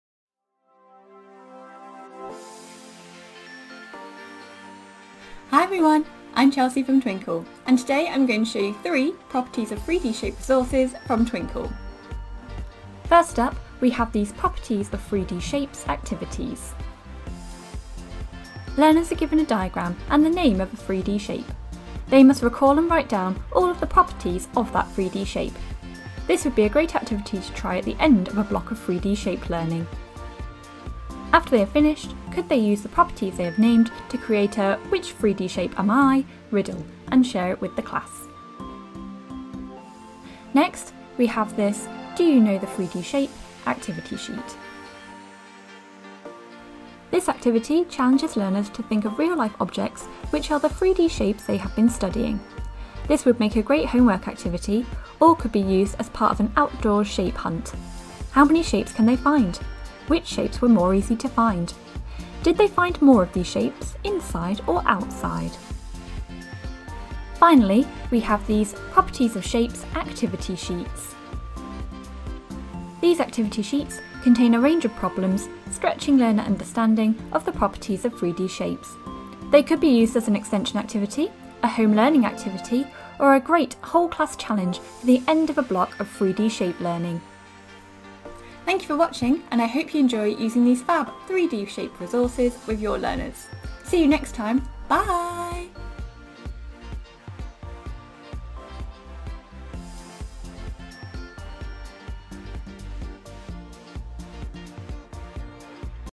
How to Use Twinkl's Properties of 3D Shape Resources
In this video, Chelsey will demonstrate three of Twinkl's most-downloaded 3D Shape resources Included are lesson worksheets, maths homework activities and fun maths shape hunt lesson resources too.

0:00 Introduction
0:13 Activity 1 - Properties of 3D Shapes Activities
0:39 Activity 2 - 'Do you know the 3D shape?' activity sheet
1:07 Activity 3 - Properties of Shapes Activity Sheets
1:29 Thank you for watching